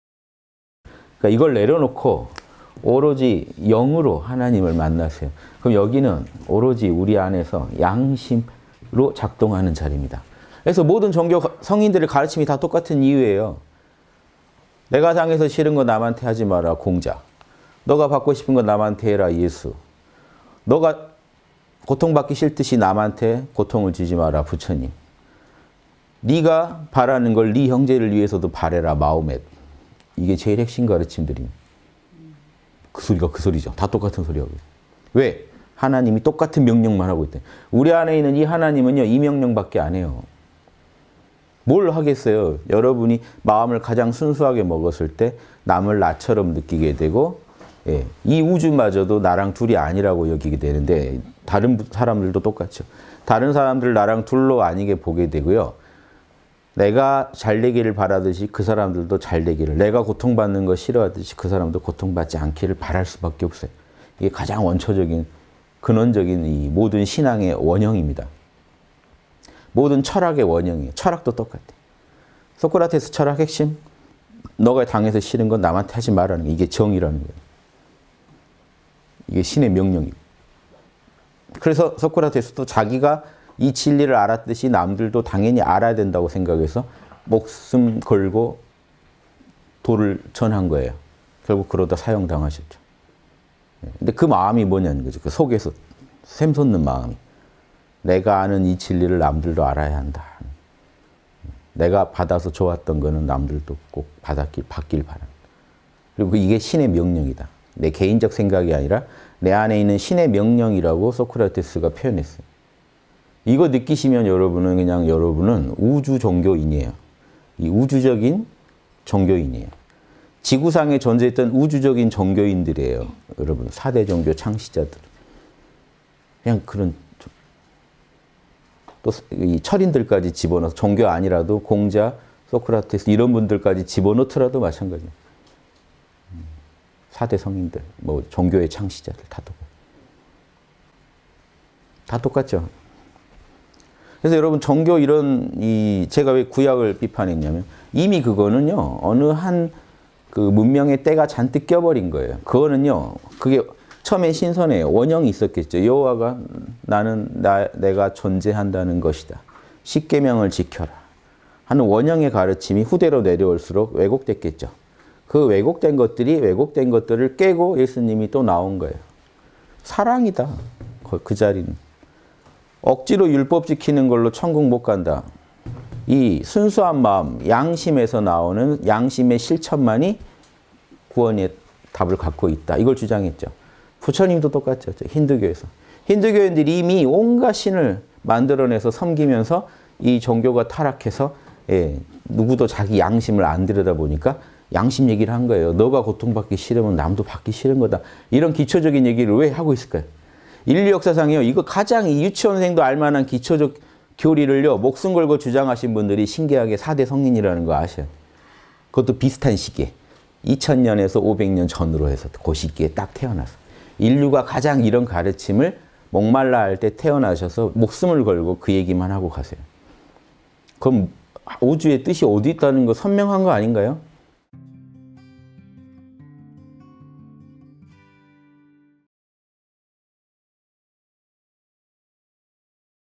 [3분 인문학] 모든 종교의 가르침이 같은 이유 _홍익학당.윤홍식
(171029 일요 대담) 자, 이걸 내려놓고 오로지 영(靈)으로 하느님을 만나세요.
그럼 여기는, 오로지 우리 안에서 양심으로 작동하는 자리입니다.
그래서 모든 종교 성인들의 가르침이 다 똑같은 이유예요.

‘내가 당해서 싫은 것 남한테 하지 마라.’-공자
‘네가 갖고 싶은 것 남한테 해라.’-예수
‘네가 고통받기 싫듯이 남한테 고통을 주지 마라.’-부처님
‘네가 바라는 걸 네 형제를 위해서도 바라라.’-마호메트
이게 제일 핵심 가르침들입니다.
그 소리가 그 소리죠. 다 똑같은 소리 하고 있습니다.

왜? 하느님이 똑같은 명령만 하고 있다는 거예요.
우리 안에 있는 이 하느님은 이 명령밖에 안 해요.
뭘 하겠어요?
여러분이 마음을 가장 순수하게 먹었을 때
남을 나처럼 느끼게 되고,
이 우주마저도 나랑 둘이 아니라고 여기게 되는데,
다른 사람들도 똑같죠.

다른 사람들도 나랑 둘로 아니게 보게 되고,
내가 잘 되기를 바라듯이 그 사람들도 잘 되기를,
내가 고통받는 것 싫어하듯이 그 사람도 고통받지 않기를
바랄 수밖에 없어요.
이게 가장 원초적인, 근원적인 모든 신앙의 원형입니다.

모든 철학의 원형이에요. 철학도 똑같아요.
소크라테스 철학 핵심,
“네가 당해서 싫은 건 남한테 하지 마라.”
이게 정의라는 거예요.
이게 신의 명령이고요.

그래서 소크라테스도 자기가 이 진리를 알았듯이
남들도 당연히 알아야 한다고 생각해서
목숨 걸고 도를 전한 거예요.
결국, 그러다 사형당하셨죠.

근데 그 마음이 뭐냐는 거죠. 그 속에서 샘솟는 마음이.
내가 아는 이 진리를 남들도 알아야 한다.
내가 받아서 좋았던 건 남들도 꼭 받길 바란다.
그리고 이게 신의 명령이다.
내 개인적 생각이 아니라 내 안에 있는 신의 명령이라고
소크라테스가 표현했어요.

이거 느끼시면 여러분은 그냥 우주 종교인이에요.
우주적인 종교인이에요.
지구상에 존재했던 우주적인 종교인들이에요, 여러분
4대 종교 창시자들은.

철인들까지 집어넣어서 종교 아니라도
공자, 소크라테스 이런 분들까지 집어넣더라도
마찬가지예요.
4대 성인들, 종교의 창시자들 다 똑같죠.
다 똑같죠?
그래서 여러분 제가 왜 구약을 비판했냐 하면
이미 그거는요, 어느 한 문명의 때가 잔뜩 껴 버린 거예요.

그게 처음엔 신선해요. 원형이 있었겠죠.
여호와가 “나는 내가 존재한다는 것이다.”
“십계명을 지켜라!”
한 원형의 가르침이 후대로 내려올수록 왜곡됐겠죠.
그 왜곡된 것들이 왜곡된 것들을 깨고
예수님이 또 나온 거예요.

“사랑이다, 그 자리는.”
“억지로 율법 지키는 거로 천국 못 간다.“
“이 순수한 마음, 양심에서 나오는 양심의 실천만이
구원의 답을 갖고 있다.” 이걸 주장했죠.

부처님도 똑같죠, 힌두교에서.
힌두교인들이 이미 온갖 신을 만들어내서 섬기면서
이 종교가 타락해서 누구도 자기 양심을 안 들여다보니까
양심 얘기를 한 거예요.
“네가 고통받기 싫으면 남도 고통받기 싫은 거다.”
이런 기초적인 얘기를 왜 하고 있을까요?

인류 역사상 가장 유치원생도 알만한 기초적 교리를
목숨 걸고 주장하신 분들이 신기하게
4대 성인이라는 걸 아셔야 해요.
그것도 비슷한 시기에.

2000년(전)에 500년 전후로 해서 그 시기에 딱 태어나서,
인류가 가장 이런 가르침을 목말라 할 때 태어나셔서
목숨을 걸고 그 얘기만 하고 가세요.
그럼 우주의 뜻이 어디 있다는 건지
선명한 거 아닌가요?